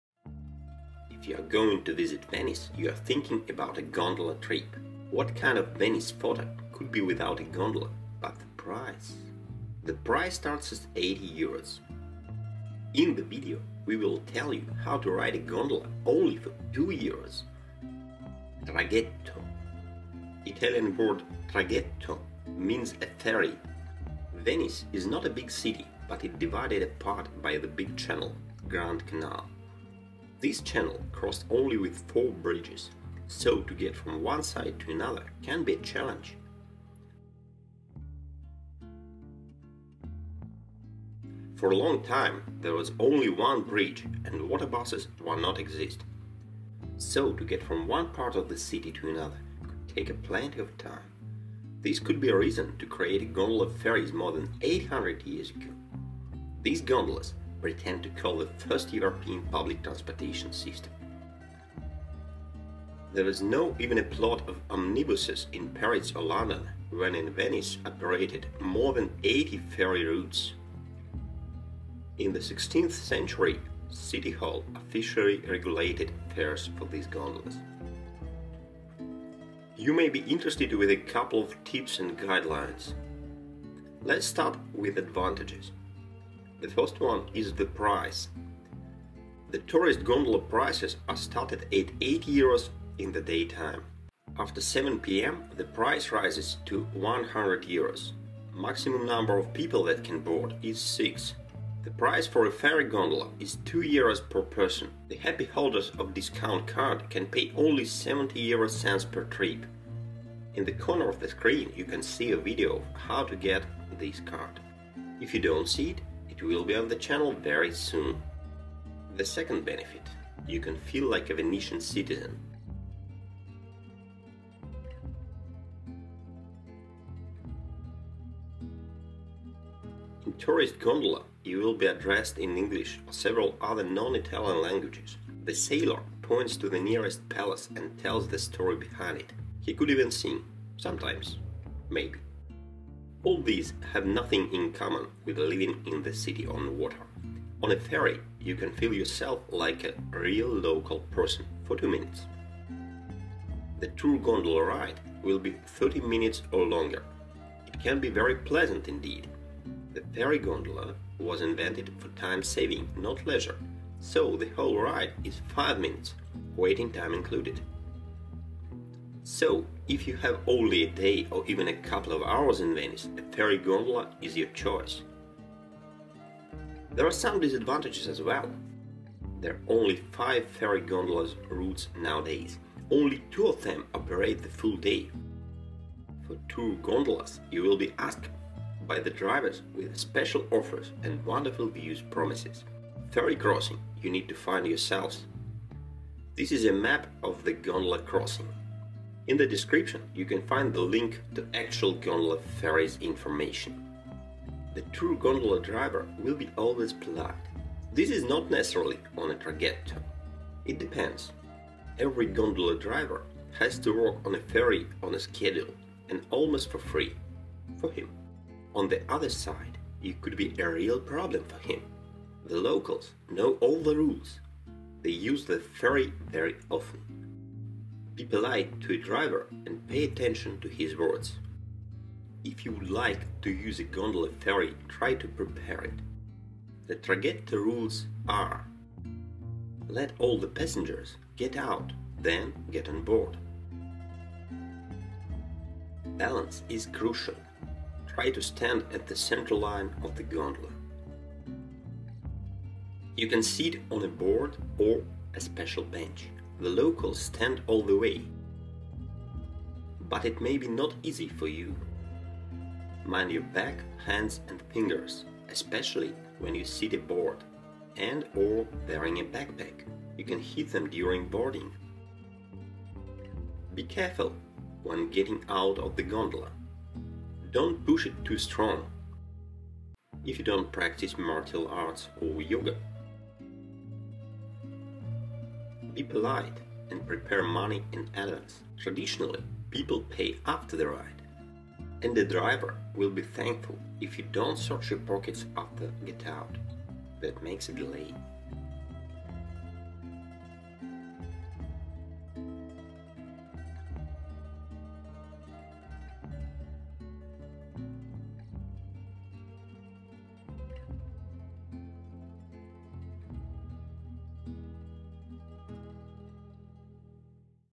Gondola ferry. Traghetto. How to ride a gondola for 2 euros.
From this video, you will know about gondola ferries over Grand Canal, pricing, and rules.